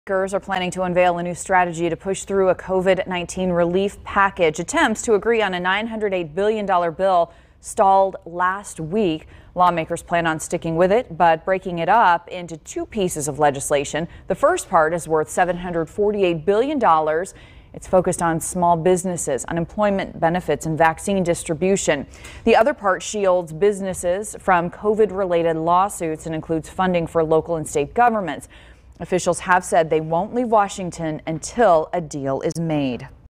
COVID-19 relief bill talks
Democrats and Republicans continue to work to find middle ground in COVID-19 relief.